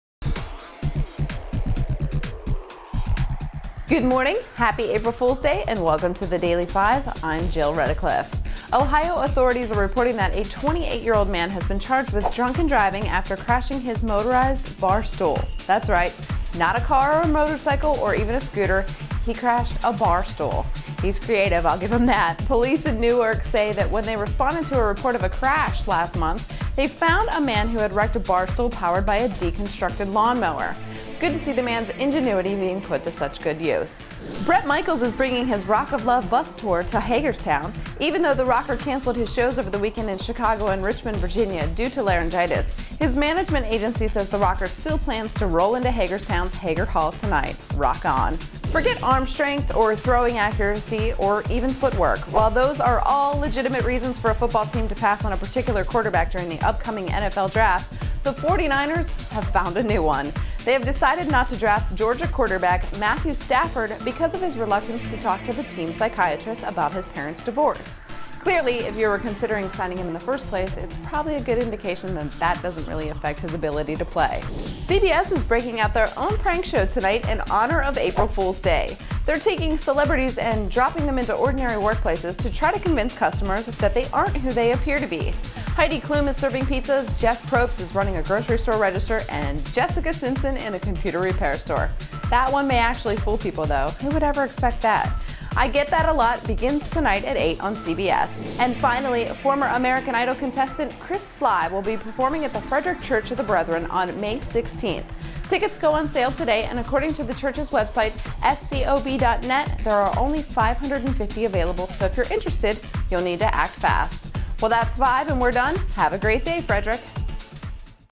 Daily Five 04/01/09
Crashing a bar stool?; Bret Michaels hits Hagerstown; NFL prospect rejected for what?